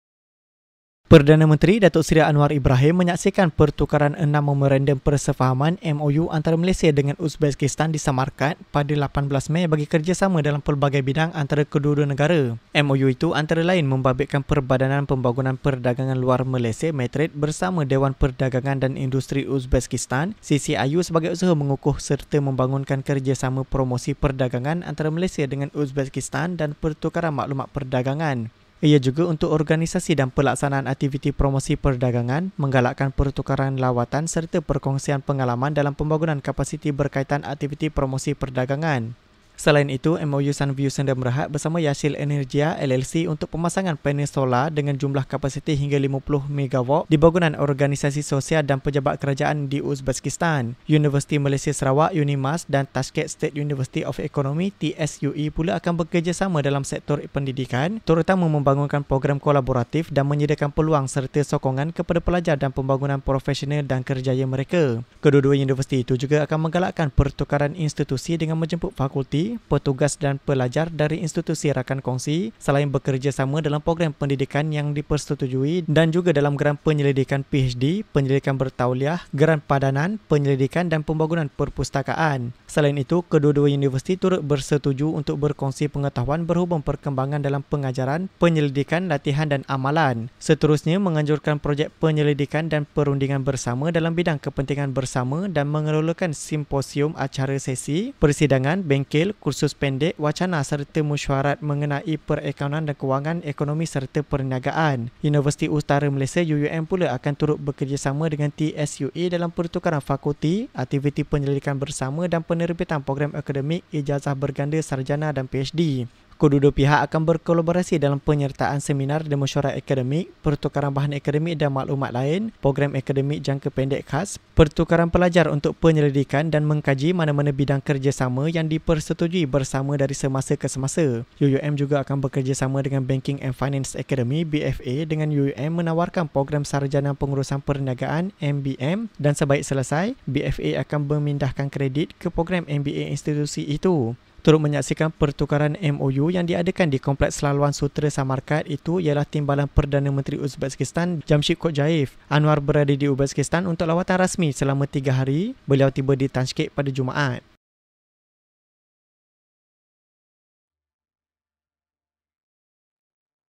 Anwar saksi pertukaran 6 MoU Malaysia dan Uzbekistan
Perdana Menteri, Datuk Seri Anwar Ibrahim, menyaksikan pertukaran enam memorandum persefahaman (MoU) antara Malaysia dengan Uzbekistan, di Samarkand, pada 18 Mei, bagi kerjasama dalam pelbagai bidang antara kedua-dua negara.